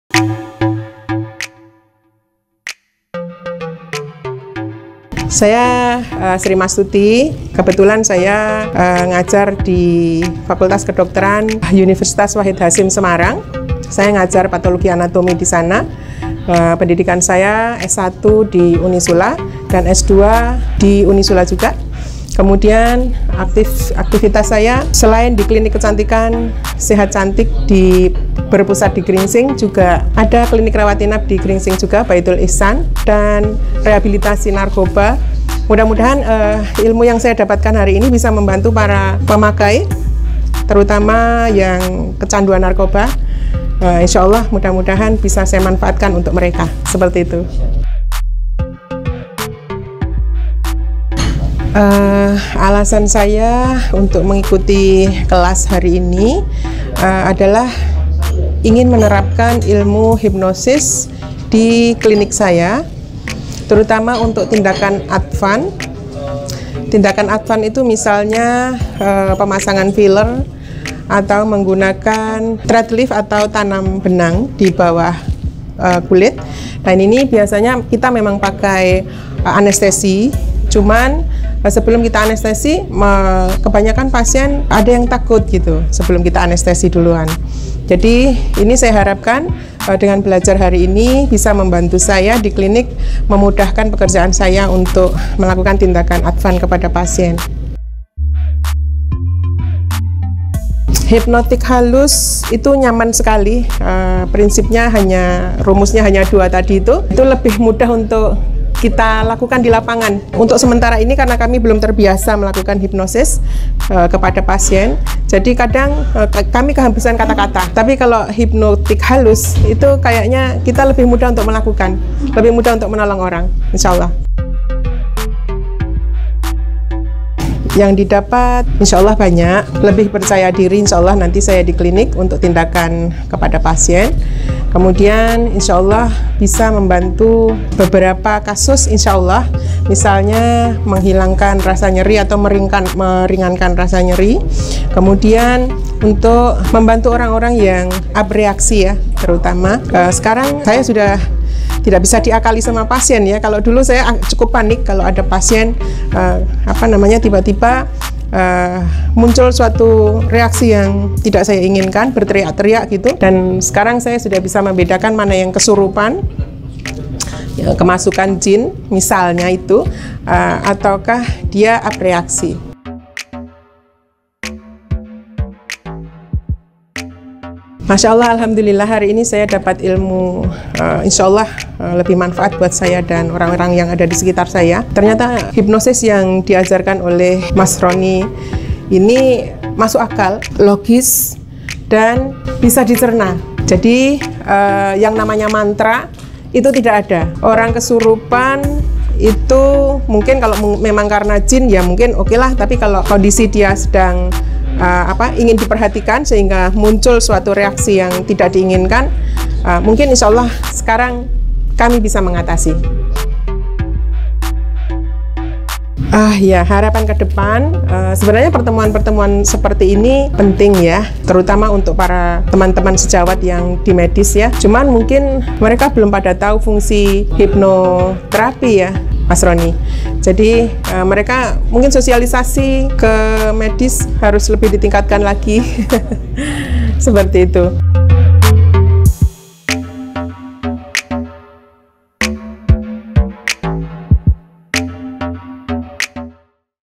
TANGGAPAN DOKTER SRI MASTUTI , M.BIOMED TENTANG HIPNOTIS HALUS
Workshop Master hipnotis halus di Jakarta tanggal 27 maret 2022 dihadiri oleh pasangan dokter  Zamroni Ali dan dokter Sri Mastuti dari Batang jawa tengah, dan beginilah kesan pesan beliau setelah mengikuti kelas hipnotis halus di Jakarta kemaren

ORDER KITAB DESENDRIA - MASTERING OF HIPNOTIS HALUS